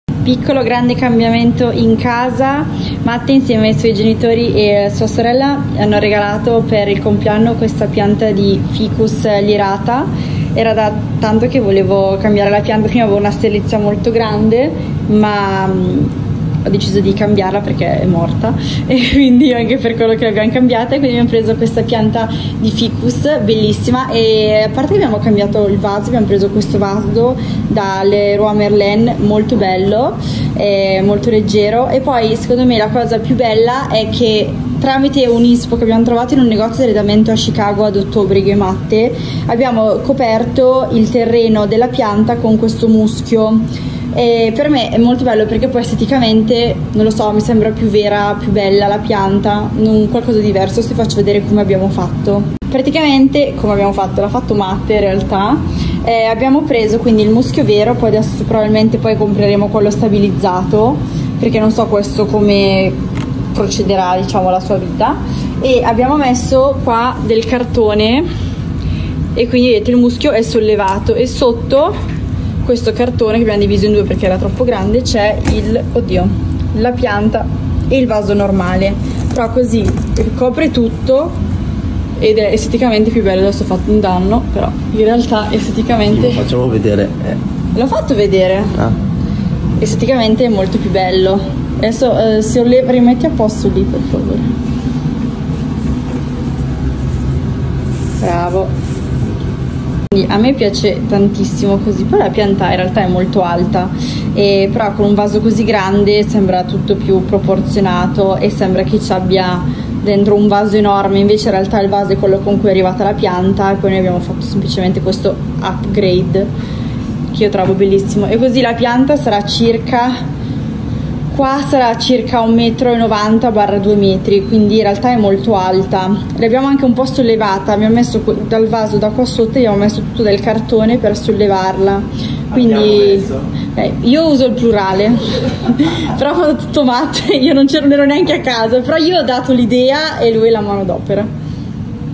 Valentina ferragni - L'elemento d'arredo che rende la casa più elegante
Valentina Ferragni mostra l'ultimo elemento d'arredo che le è stato regalato per la casa che appare ancora più chic ed elegante